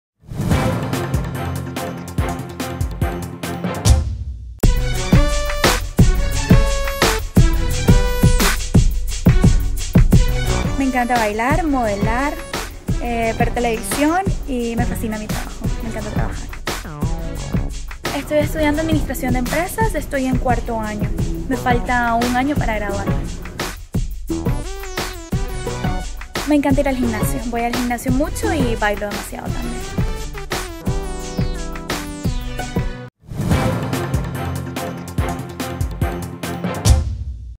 Musa: la bella Isalylliam Almendárez
Isalylliam Almendárez posó para la cámara de El Nuevo Diario y contó cuáles son sus intereses y qué hace para mantenerse en forma.